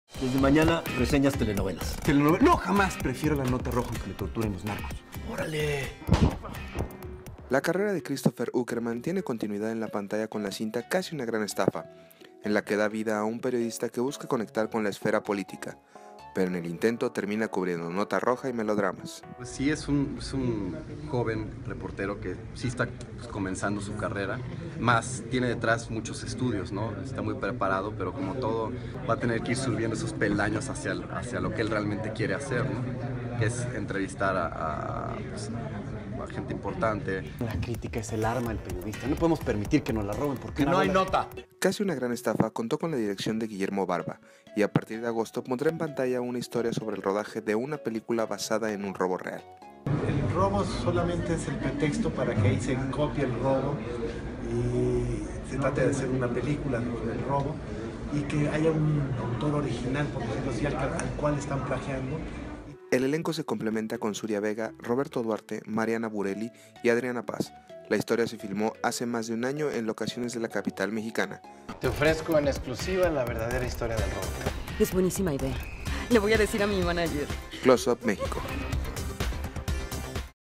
Uckermann da vida a un periodista
Christopher Uckermann protagoniza Casi una gran estafa, comedia producida por Itaca Films y Papalote Films.